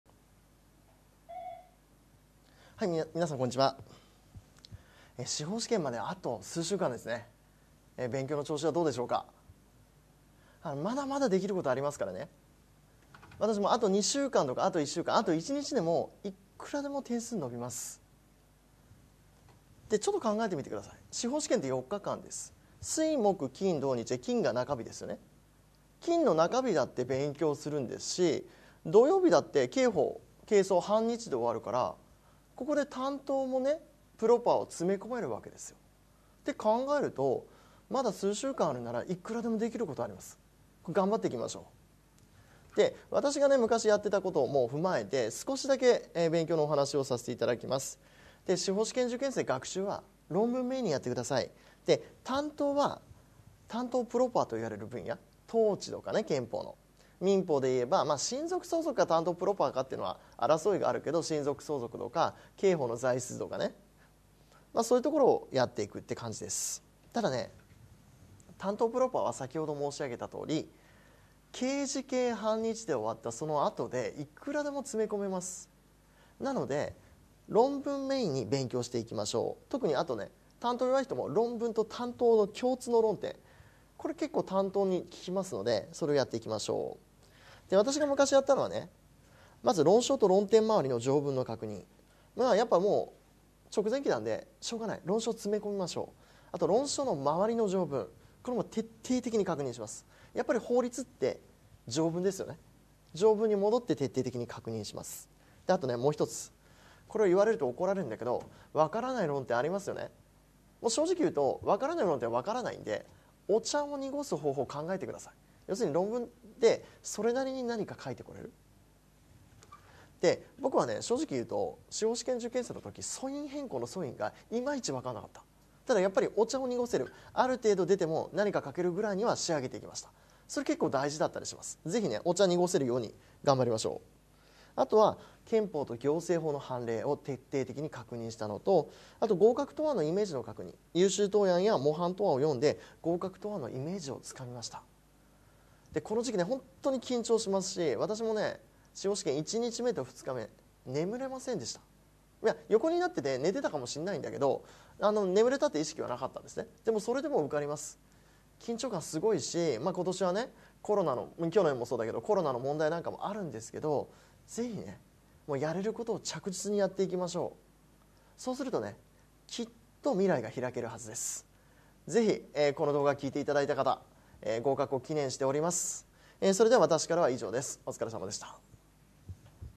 【LEC司法試験】令和3年5月本試験 受験生応援メッセージ～武山茂樹ＬＥＣ専任講師～
令和3年5月実施の司法試験に向けて、ラストスパートをかける受験生の皆様へ、講師からの応援メッセージをお届けします！